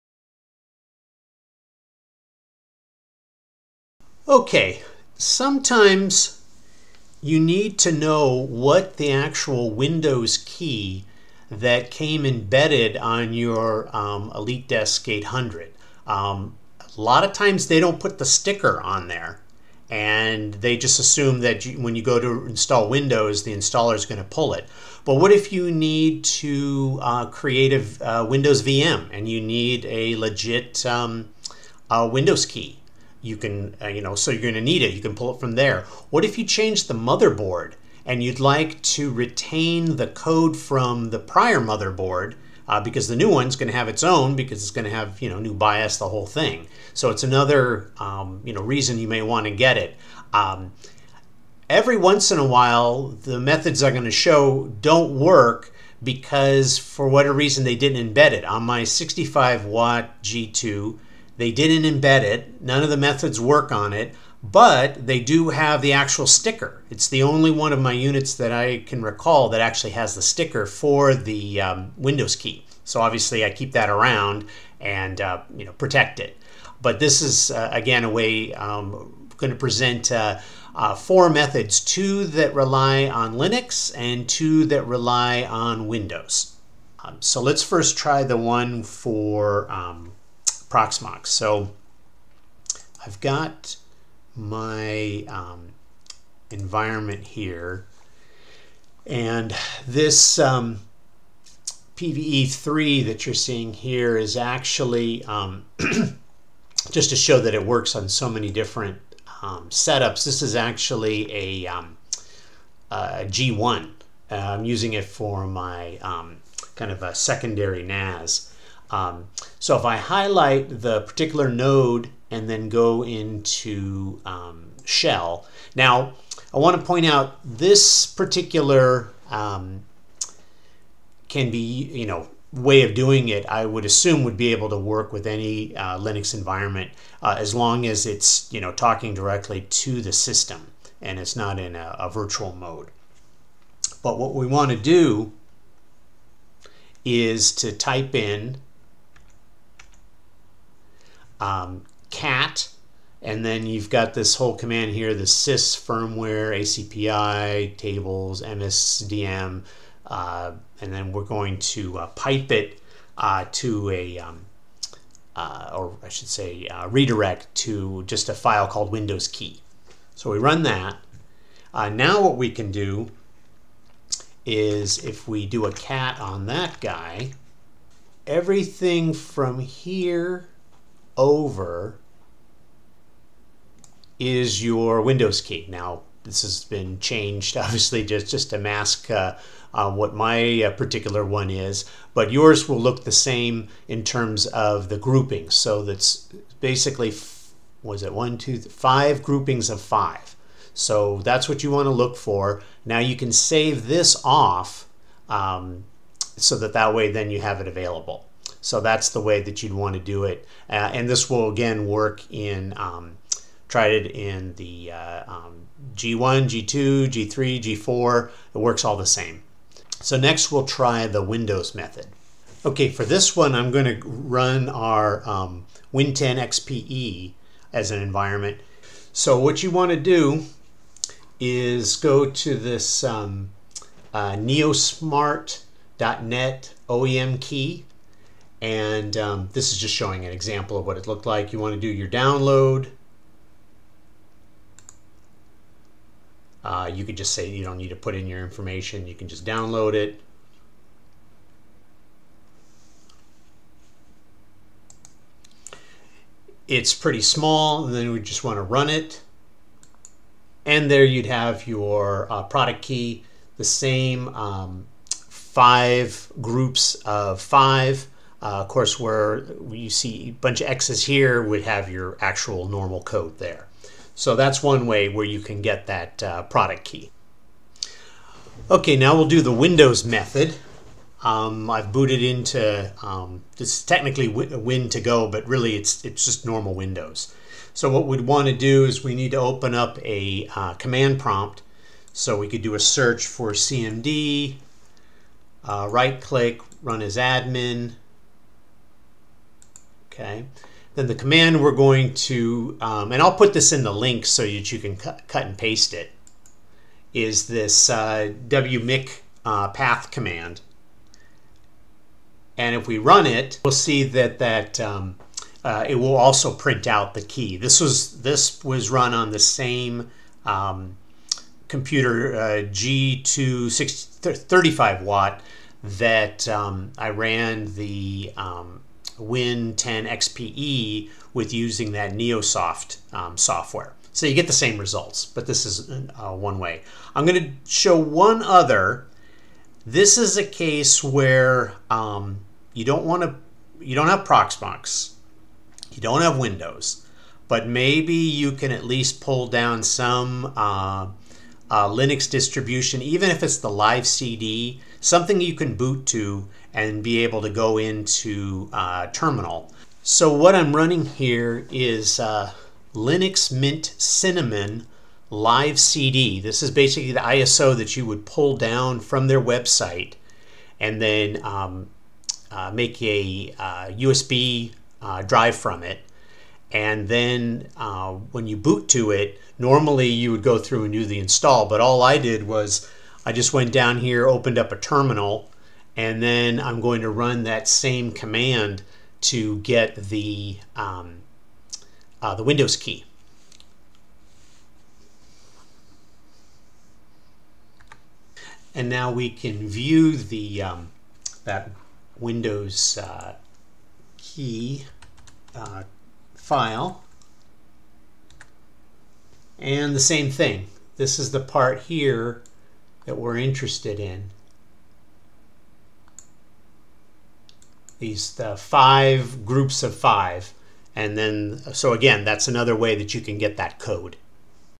Find that Windows Key on an EliteDesk 800 G1-G5 Mini
Sometimes you need to know the Windows Key that came embedded on your HP EliteDesk 800 G1-G5 Mini.  Four methods of retrieval are presented.

Proxmox commands:
cat /sys/firmware/acpi/tables/MSDM &gt windows_key
cat windows_key

While inside of WIN10XPE:
- I normally don't link to software but this was suggested on several sites - plus the WIN10XPE environment is pretty isolated - I'd only use the neosmart if WIN10XPE was your only option

While inside Linux terminal
sudo cat /sys/firmware/acpi/tables/MSDM &gt windows_key
cat windows_key